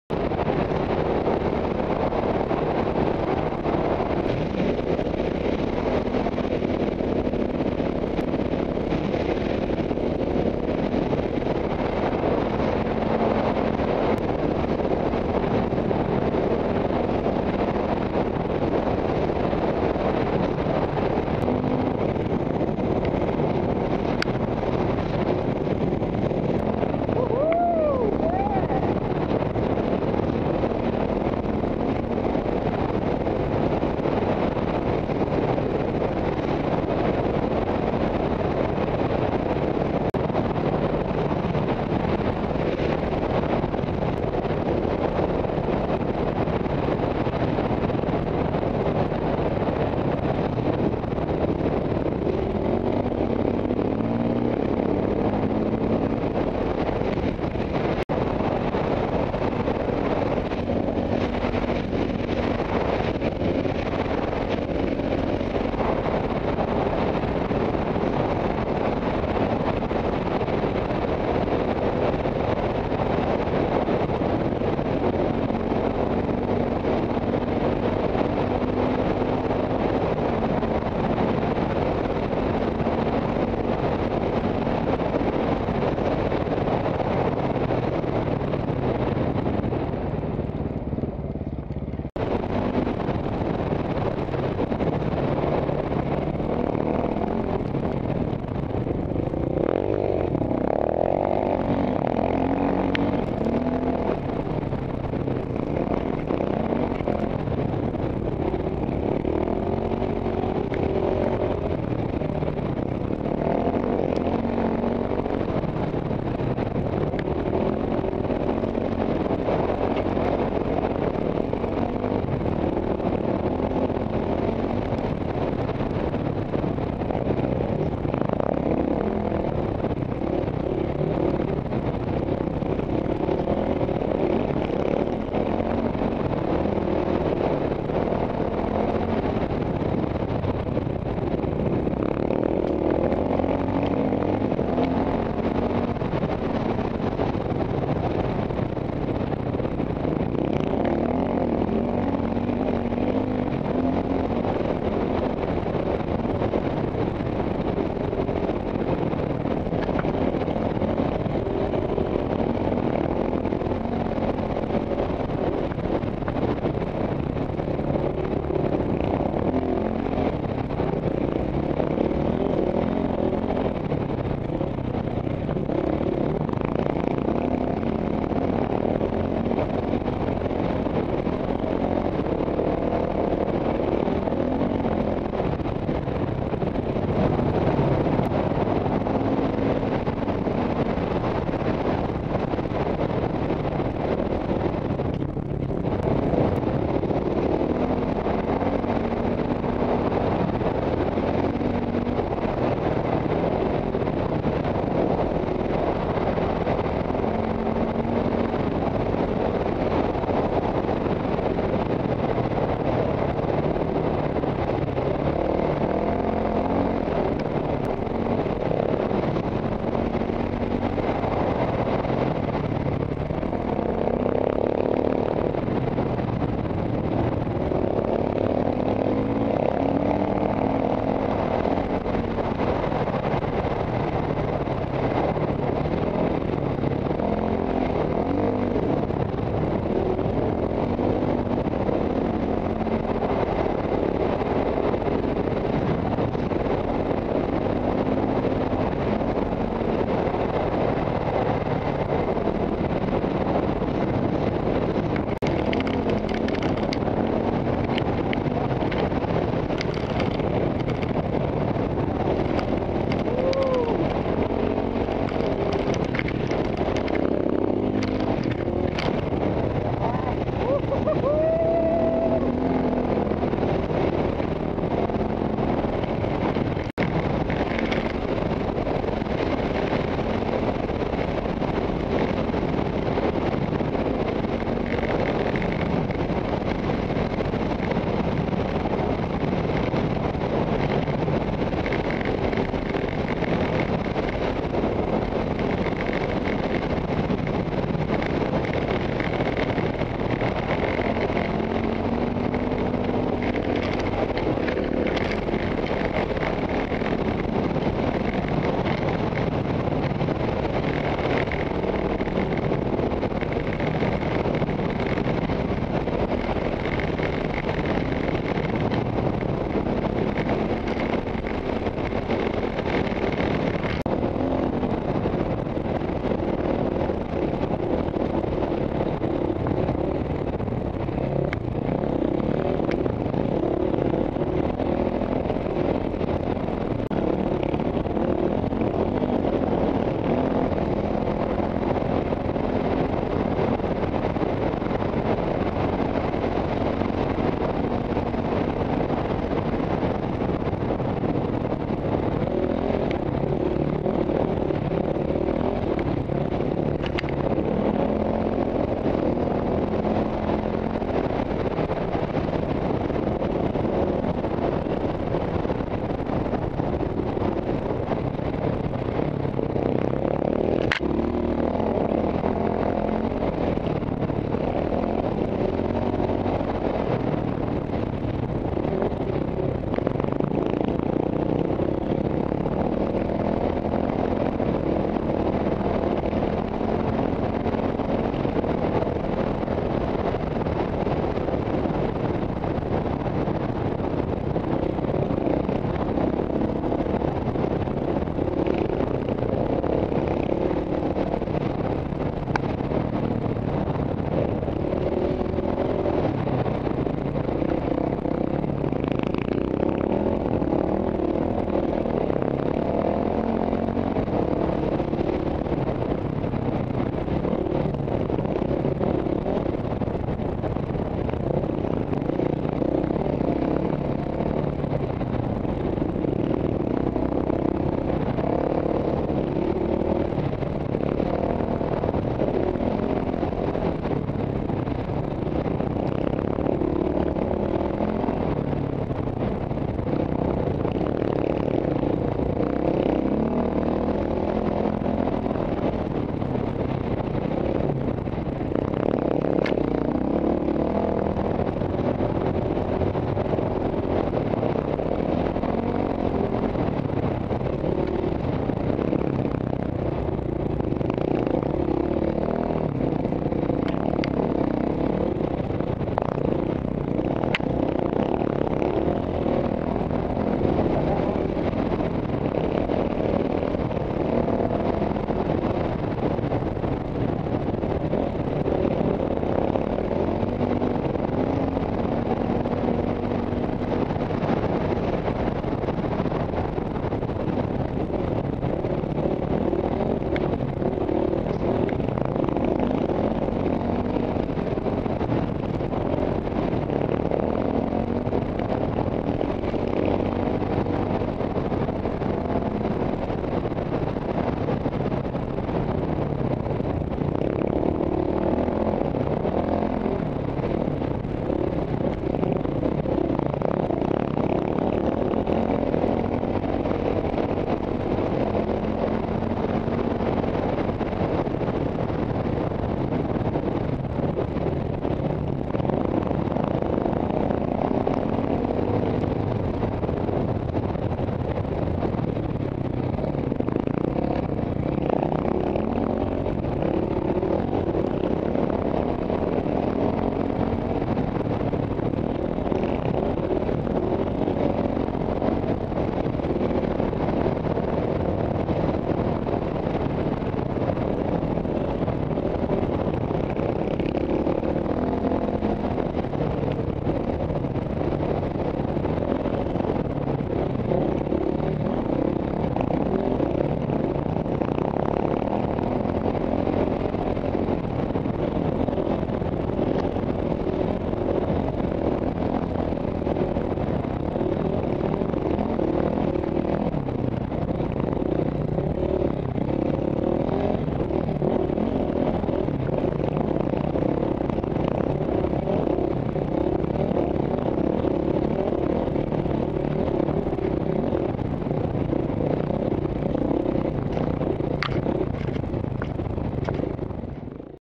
Ouyen 2014 Day 1 2nd bit
Next bit of Ouyen 2014 600k two day Event. No support staff or film crew, just hard bastards riding. Standard size tanks, spare 12 or 13 litres in the backpack, plus everything else.
Must see action.